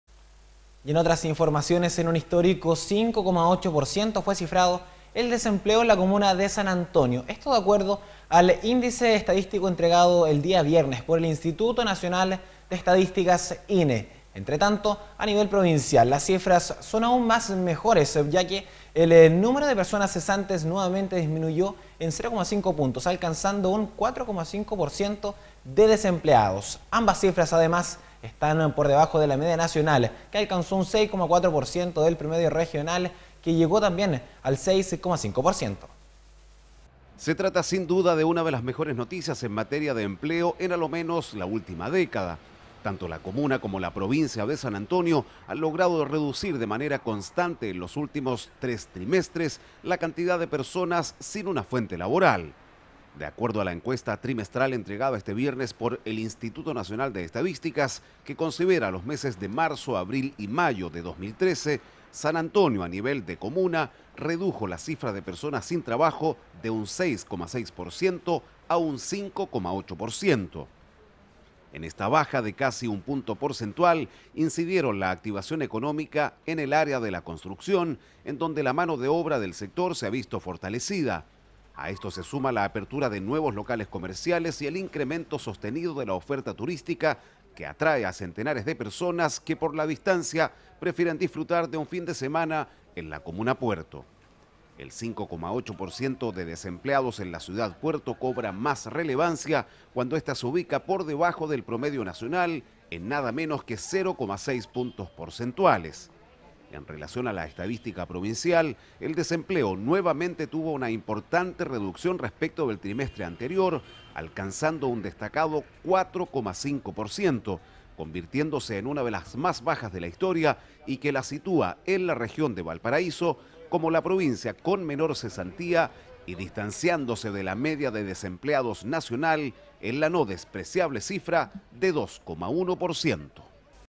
01 JUL 2013 CIFRAS DE DESEMPLEO
EN UN HISTÓRICO 5,8 POR CIENTO FUE CIFRADO EL DESEMPLEO EN LA COMUNA DE SAN ANTONIO, DE ACUERDO AL ÍNDICE ESTADÍSTICO ENTREGADO ESTE VIERNES POR EL INSTITUTO NACIONAL DE ESTADÍSTICAS INE.
ENTRE TANTO, A NIVEL PROVINCIAL LAS CIFRAS SON AUN MEJORES, YA QUE EL NÚMERO DE PERSONAS CESANTES NUEVAMENTE DISMINUYÓ EN 0,5 PUNTOS, ALCANZANDO UN 4,5 POR CIENTO DE DESEMPLEADOS.
AMBAS CIFRAS ADEMÁS, ESTÁN POR DEBAJO DE LA MEDIA NACIONAL QUE ALCANZÓ UN 6,4 POR CIENTO Y DEL PROMEDIO REGIONAL QUE LLEGÓ AL 6,5 POR CIENTO.